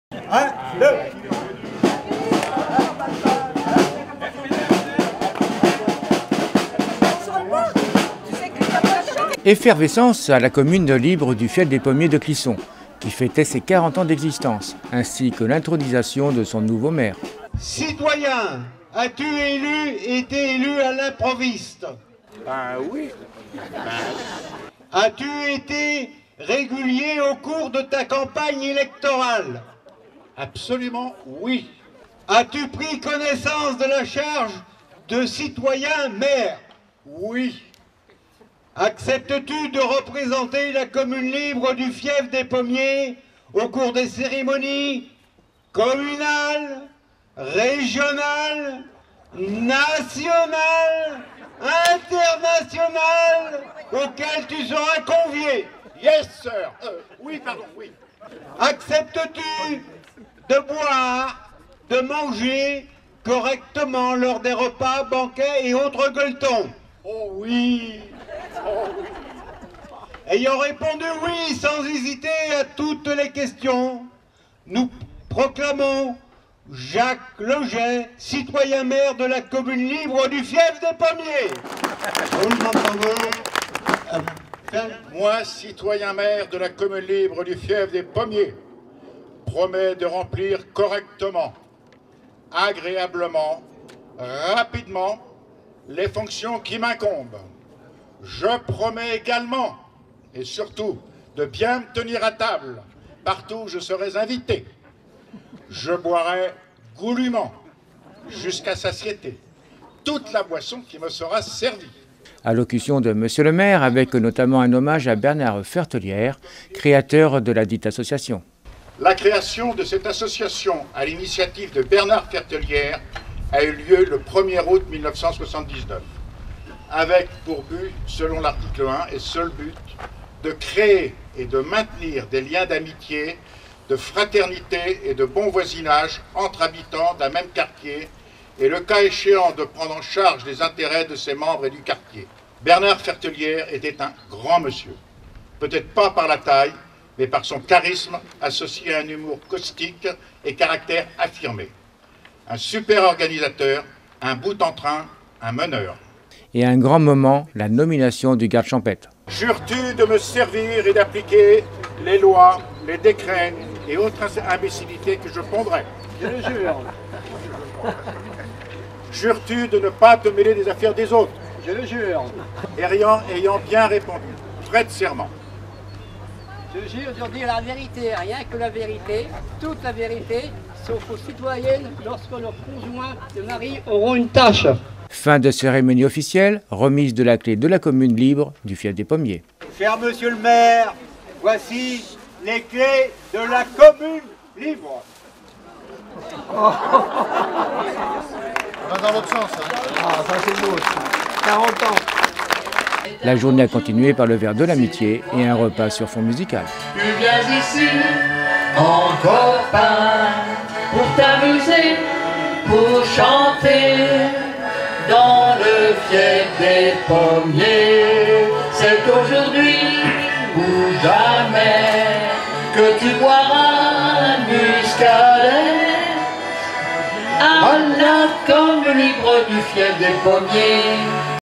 Fief des Pommiers
Effervescence à la commune libre du Fief des Pommiers, de Clisson, qui fêtait ses 40 ans d’existence ainsi que l’intronisation de son nouveau maire.